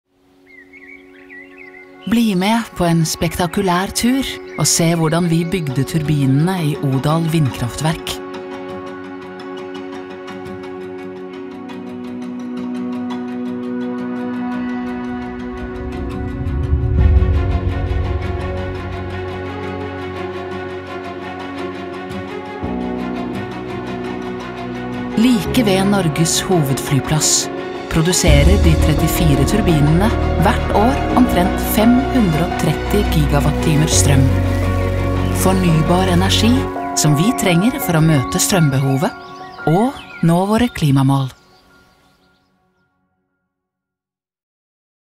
Bli med på en spektakulær tur og se hvordan vi bygde turbinene i Odal Vindkraftverk - kort versjon
Ta deg en liten pause og nyt 45 sekunder med spektakulær transport og luftige svev fra Odal Vindkraftverk 🌲

Vindkraftverket er et resultat av modig satsing og framtidsrettet ingeniørkunst. Fra vi startet monteringen av turbinene, til alle 34 sto ferdig, tok det bare ett år, og nå produserer de over en halv terawatt fornybar strøm i året – nok strøm til 33.000 boliger! 🏡⚡️

Sammen med Cloudberry Clean Energy og KLP er vi i Akershus Energi stolte av å bidra til økt produksjon av fornybar energi i Norge 💚

Turbinene er levert av Siemens Gamesa Renewable Energy.

Akershus Energi - 100 år med produksjon av ren, fornybar energi.